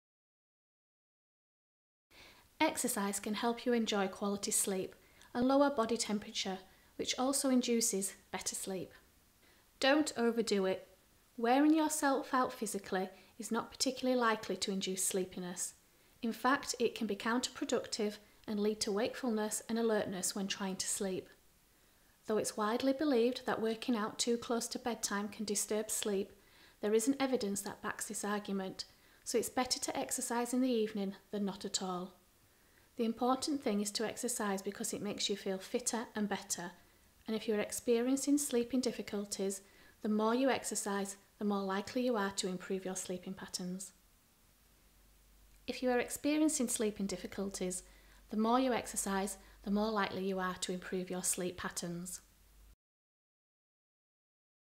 The Sleep Council: how exercise can help you get better sleep
Sports and exercise can help you to enjoy a better quality of sleep. Working out effectively can tire your body out gently, promoting a better night’s sleep. Releasing pent up tension through exercise is also highly beneficial, helping to banish stress before bedtime. Exercising also lowers your body’s temperature, which induces better sleep. However, there are several things to keep in mind when exercising to improve your sleeping habits.